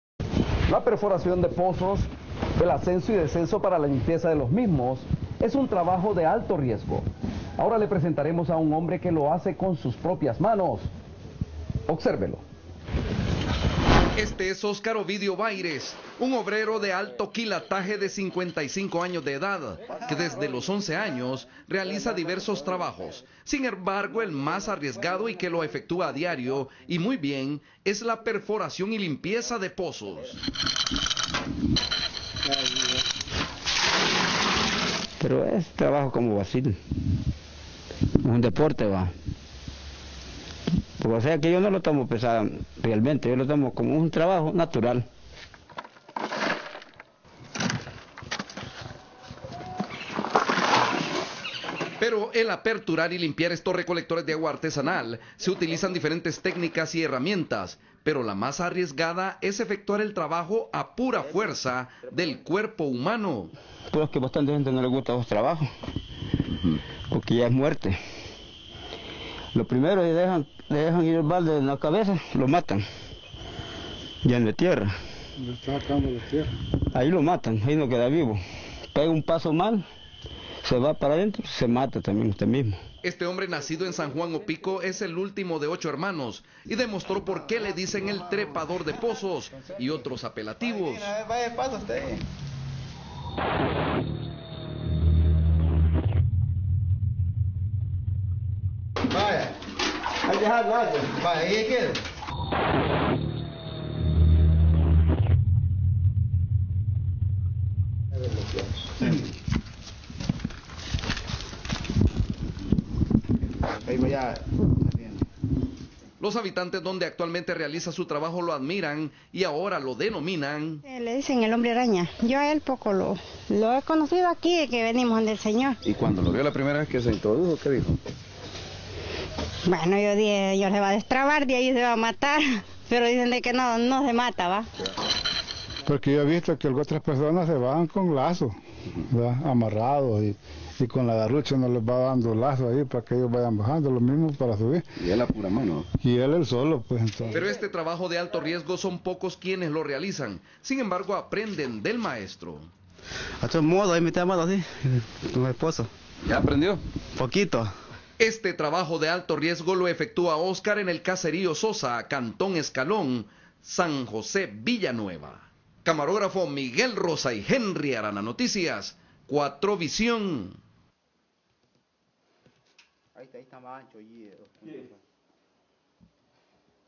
Labor de riesgo
Un hombre se gana la vida con un trabajo muy singular en el que muchas veces pone en riesgo su vida. (04/04/2013)